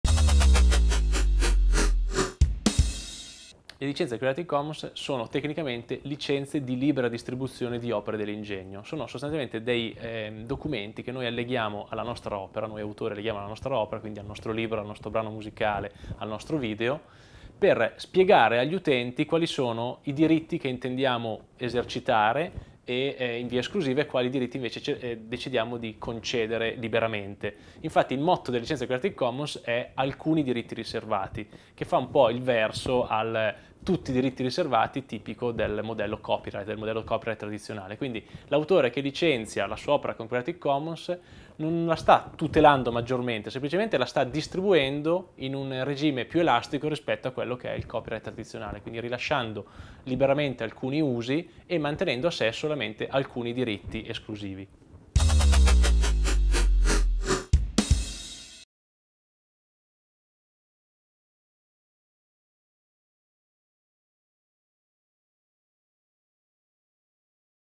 Pillole di diritto 3: Cosa sono le Creative Commons?
Simone Aliprandi ci spiega in pochi secondi che cosa sono le licejnze Creative Commons e a che cosa servono.
Un video a cura di CTRL, realizzato nell'ottobre 2015.